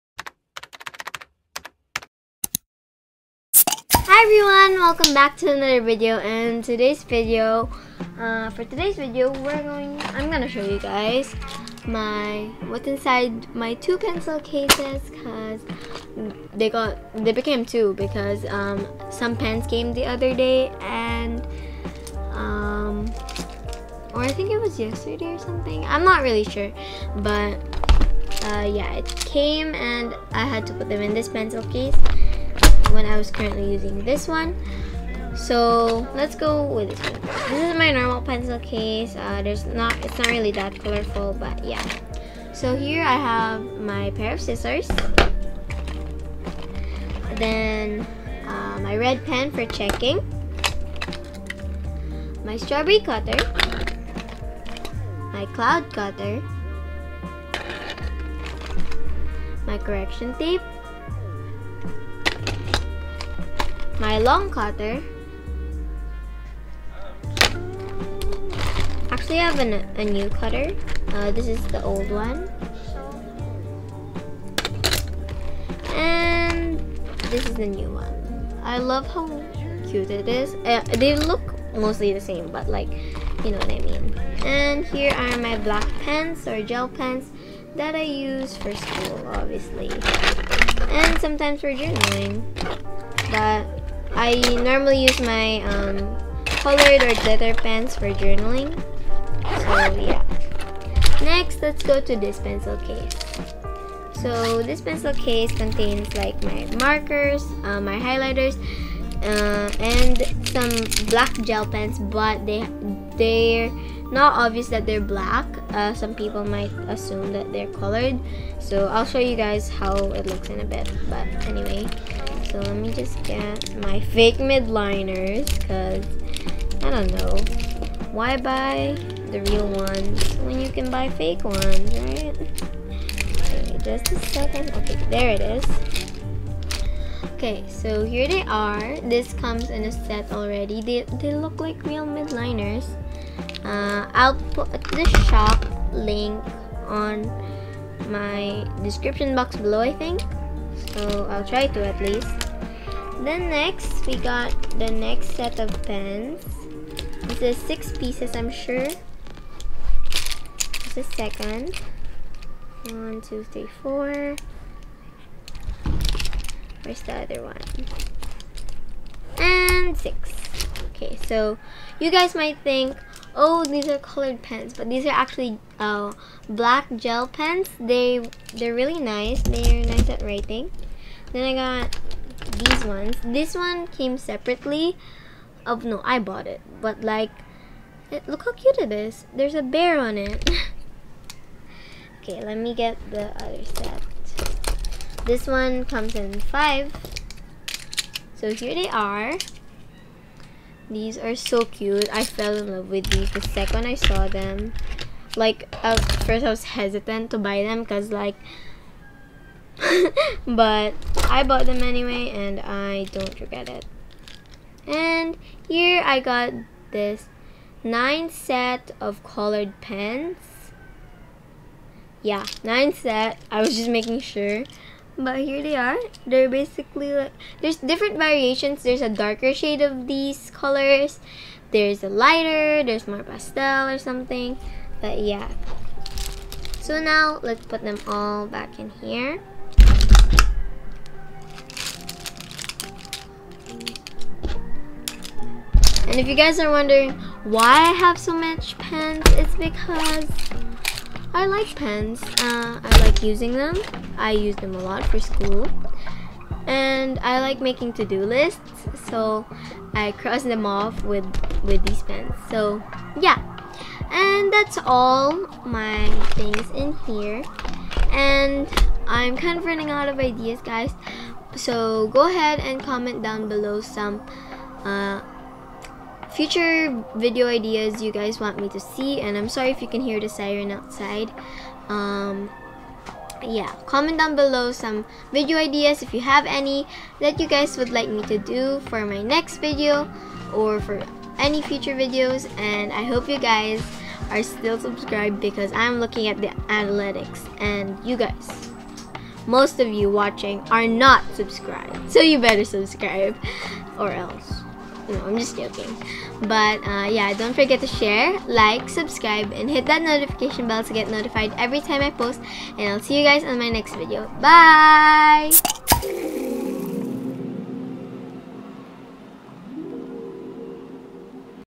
what's inside my pencil case/s 🖇🤍
hi guys! i am sorry for not posting this on my regular posting time but i hope you'll like this video about me showing what's inside my pencil cases. enjoy!!

Short QnA :

Q: What do I use to edit?
A: VLLO

Q: What do I use to film?
A: An Iphone XR !!